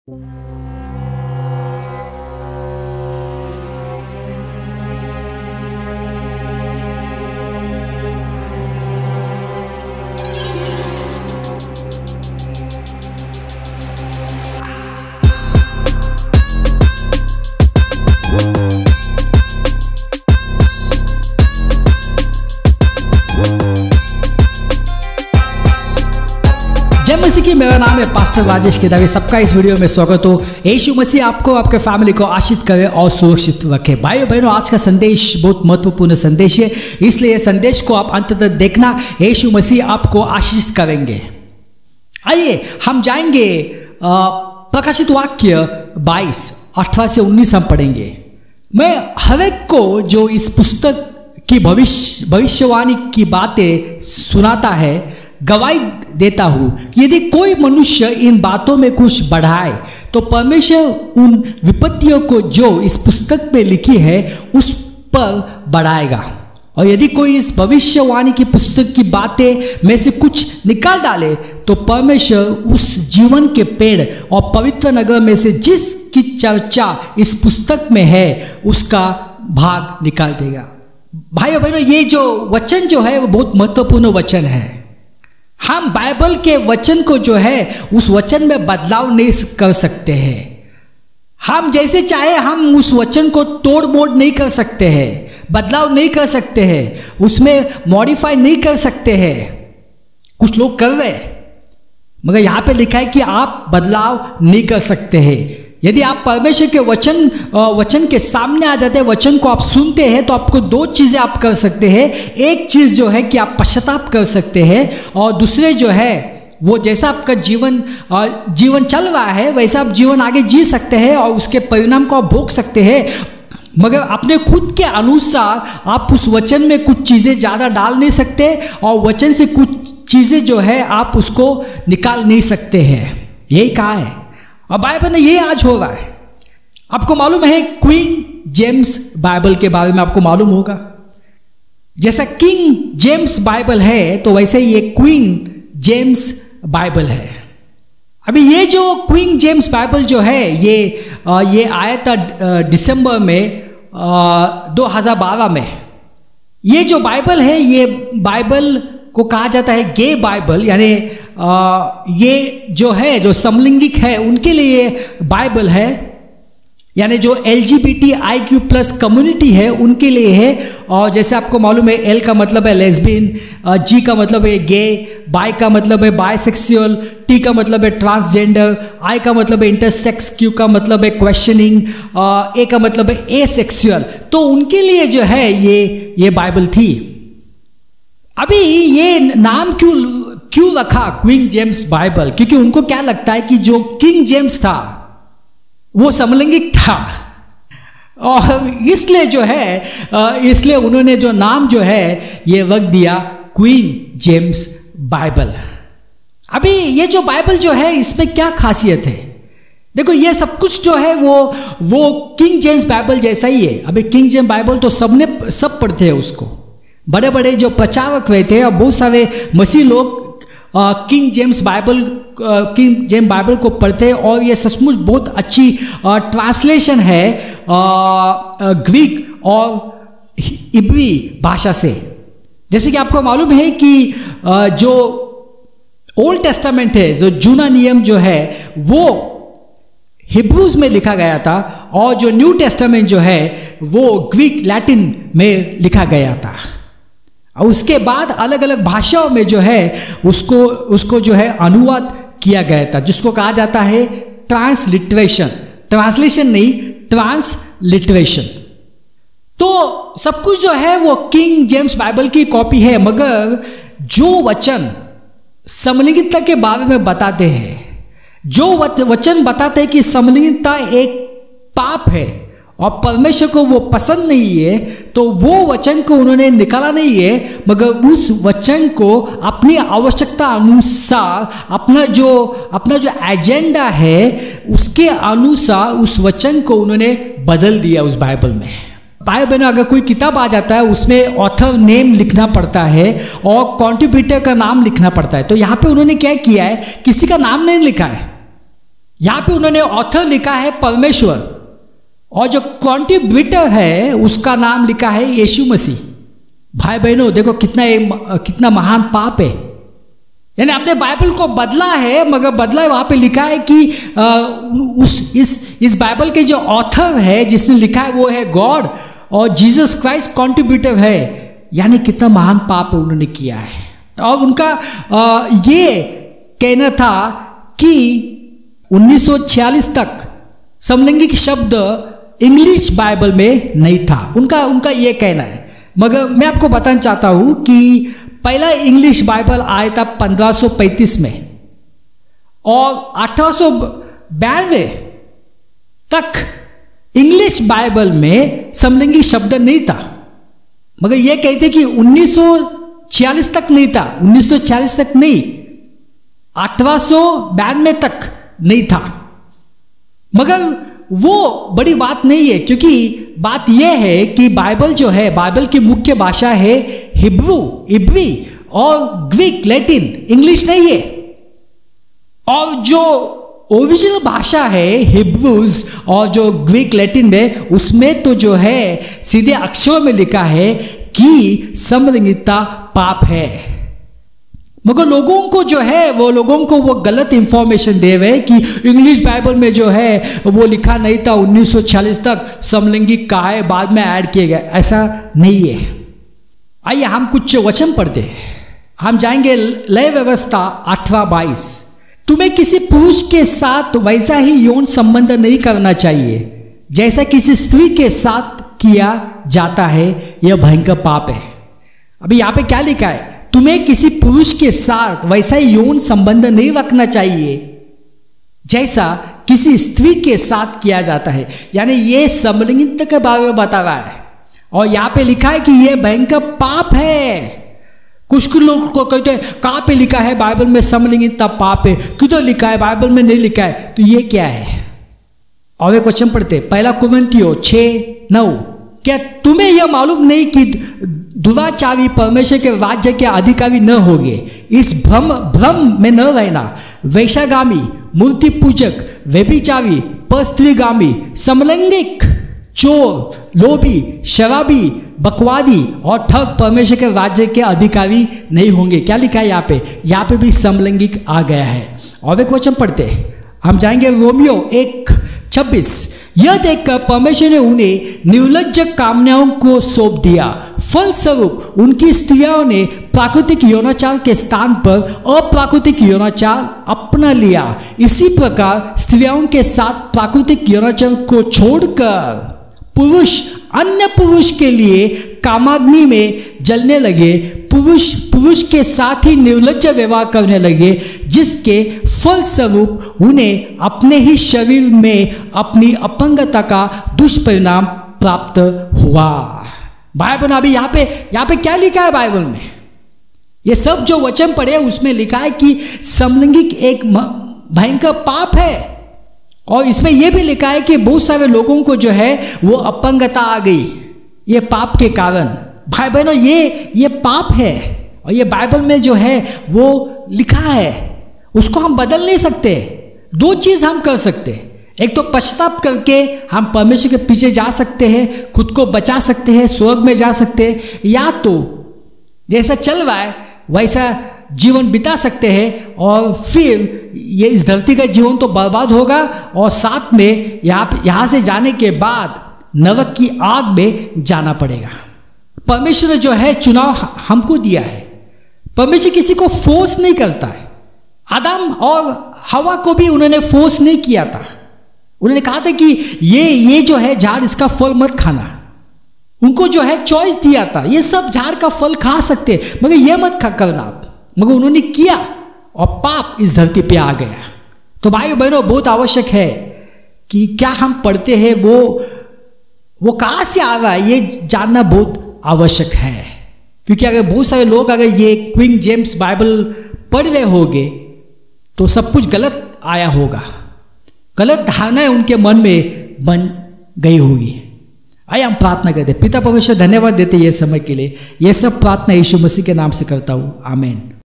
Queen James Bible || बाइबिल को भ्रष्ट करने की शैतान की योजना || Hindi Queen James Bible से बचके
About Video
"Queen James Bible" की पृष्ठभूमि रहस्यमय है। किसी प्रकाशक की पहचान नहीं की गई है, न ही किसी संपादक का नाम बताया गया है। इसके अमेज़ॅन खरीद पृष्ठ पर, लेखक को भगवान के रूप में सूचीबद्ध किया गया है, और यीशु मसीह को "योगदानकर्ता" के रूप में नामित किया गया है।

 Queen James बाइबिल का उद्देश्य क्या है?
यह अस्तित्व में क्यों है? Queen James Bible
Queen James Bible का इतिहास क्या है?